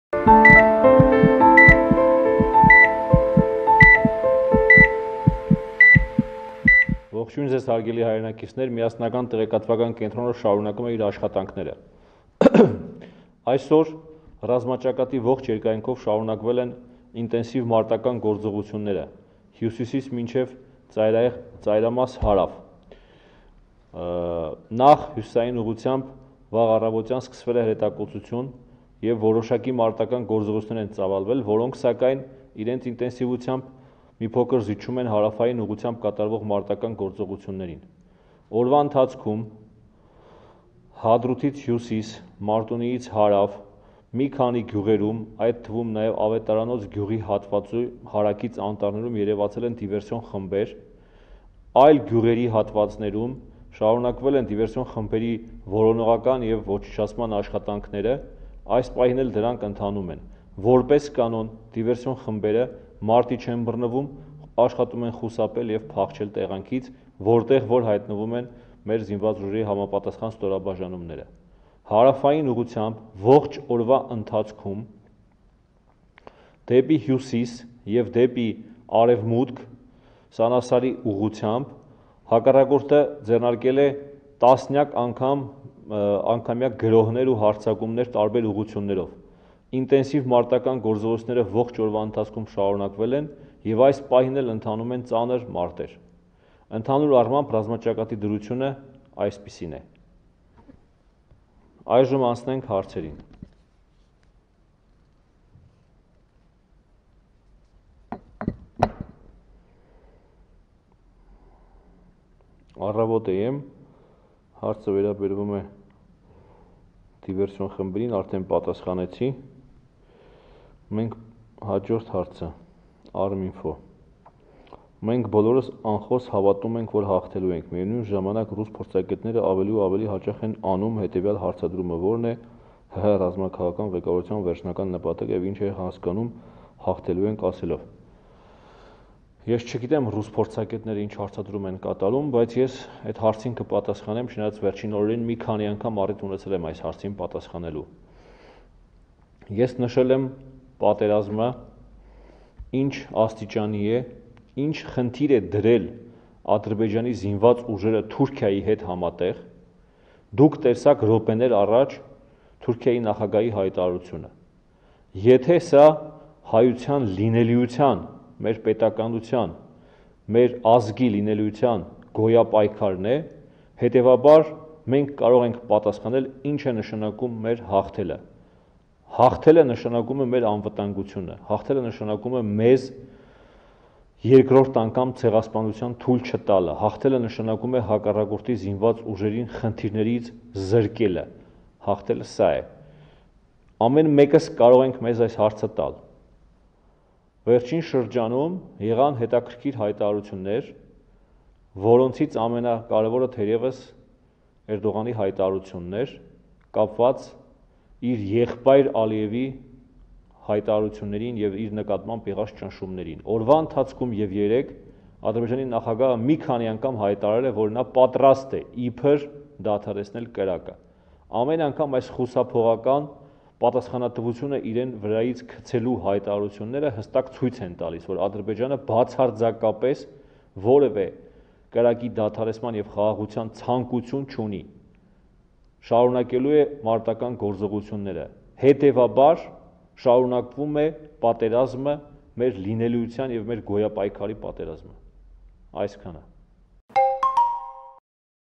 Այս պահին էլ ընթանում են ծանր մարտեր. Արծրուն Հովհաննիսյան
ՊՆ ներկայացուցիչ Արծրուն Հովհաննսիյանը ճեպազրույցին  հայտնեց, որ այսօր ռազմաճակատի ողջ երկայնքով շարունակվել են ինտենսիվ մարտական գործողությունները:
«Հյուսիսային ուղղությամբ վաղ առավոտյան սկսվել է հրետակոծություն և որոշակի մարտական գործողություններ են ծավալվել, որոնք սակայն, իրենց ինտենսիվությամբ մի փոքր զիջում են հարավային ուղղությամբ կատարվող մարտական գործողություններին»,-տեղեկացրեց Հովհանիսյանը:
Նա նաև նշեց, որ Հադրութից հյուսիս, Մարտունիից հարավ մի քանի գյուղերում, երևացել են դիվերսիոն խմբեր: